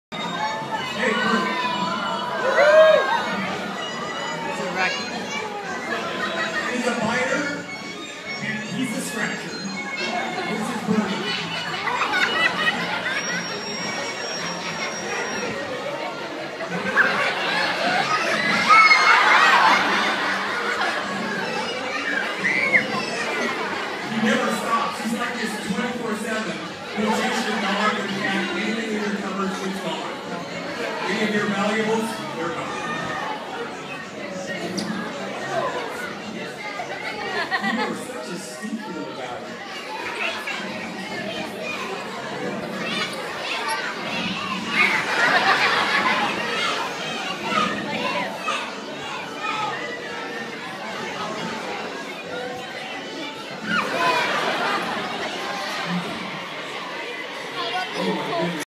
Predators of the Heart - Coati (Nasua Nasua)
Predotors of the Heart did a show at Calvary Fellowship of Mount Lake Terrace 4/27/2014.  Predators of the Heart is the Largest traveling Wildlife show in Washington States and goes to schools, events and churches.  for more information about Calvary Fellowship.